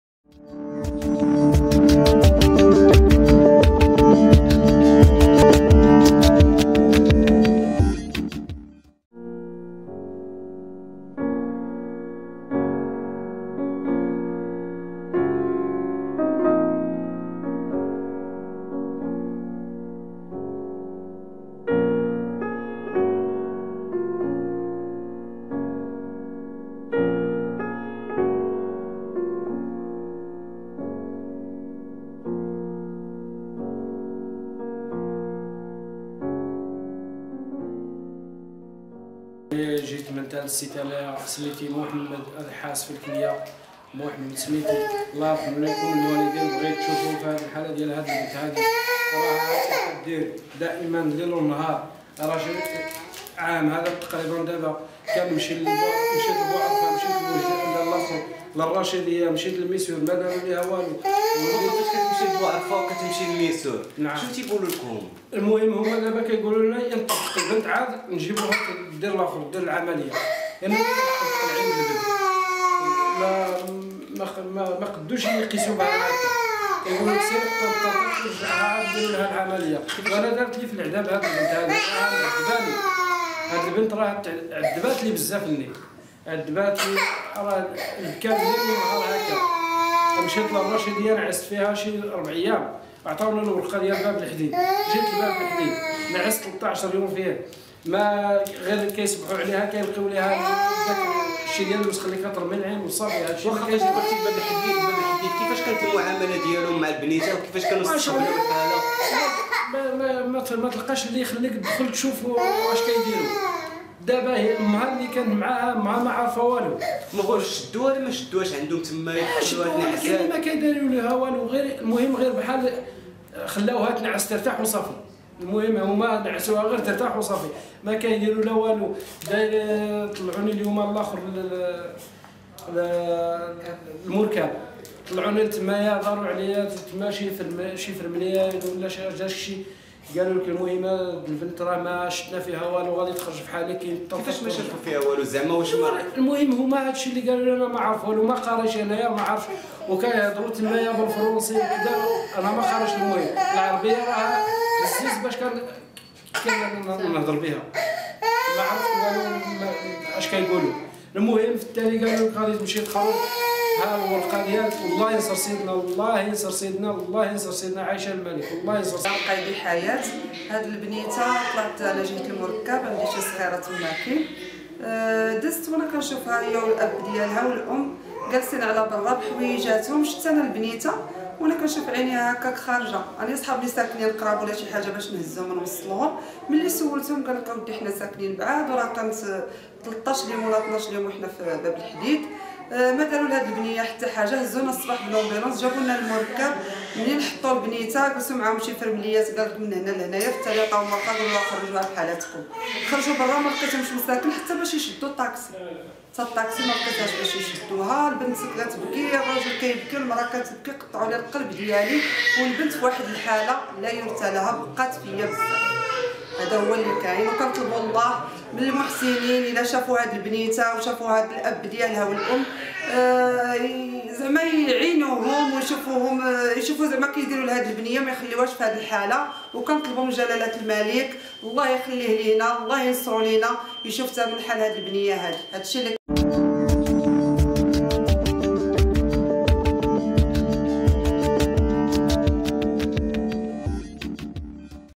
واش كندي بنتي ترتاح فالمركب ديال فاس وصافي!! راه ما كيديرو ليها والو..!! "أغثنا يا جلالة الملك"
هكذا عبر اب مكلوم عن الحالة اليئسة التي تعاني منها طفلته ولا قلب مسؤول يأخذ بيدهم..
يناشد جلالة الملك نصره الله  وذوي القلوب الرحيمة مساعدتها..